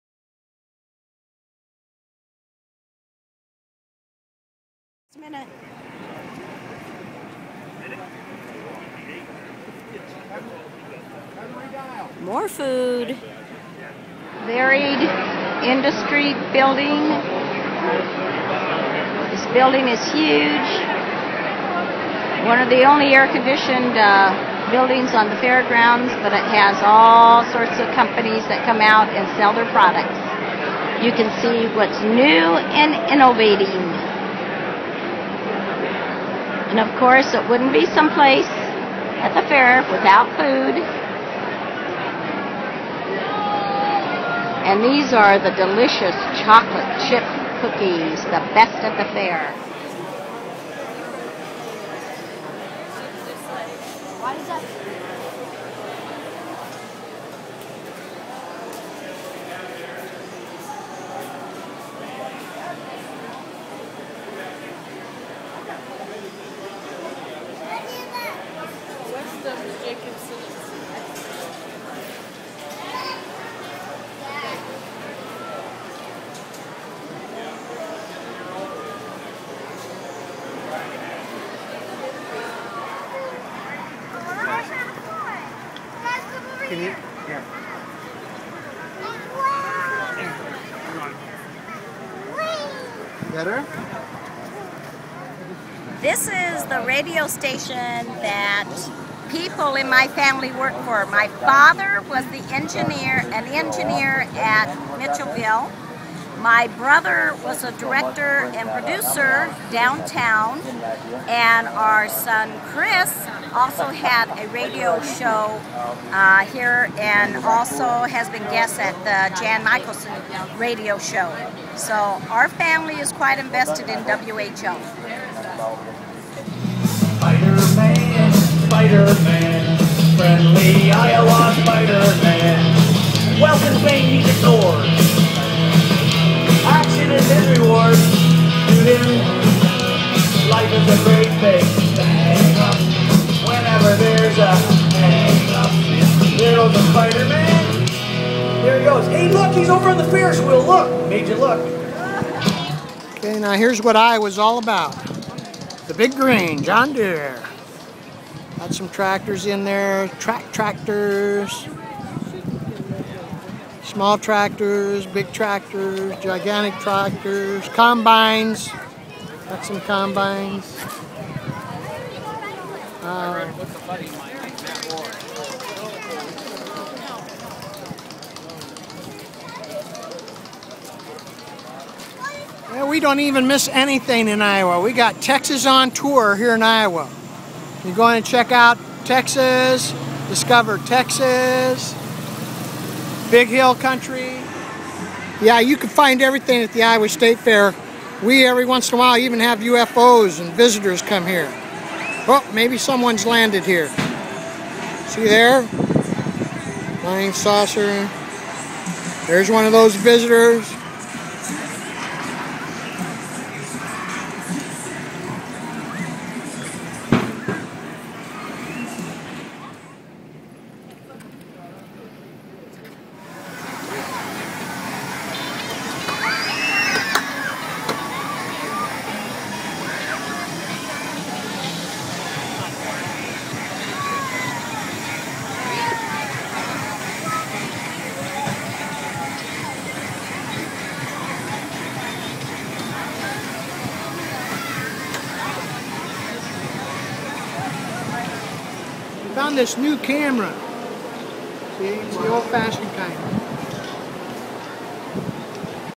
Joe Time v19 - 2012 Iowa Fair Sightings
Things one will see at the Iowa State Fair